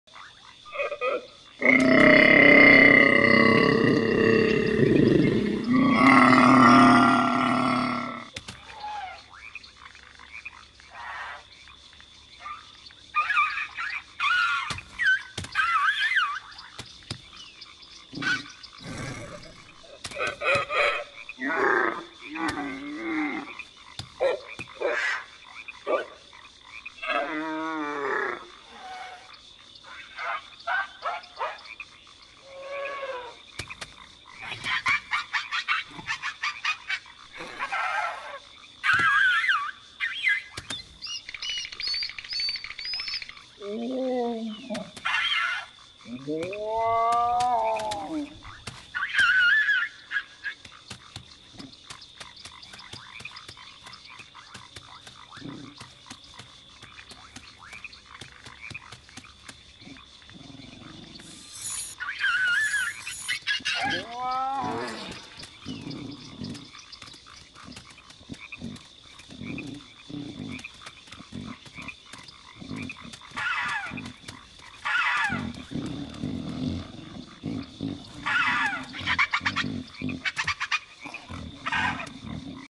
Tormented Scavenger
Tormented Scavengers scour wastelands looking for wounded prey to consume. Scavengers perfer to hunt at night. They are blind themselves, but are able to sense other spore by thier body warmth.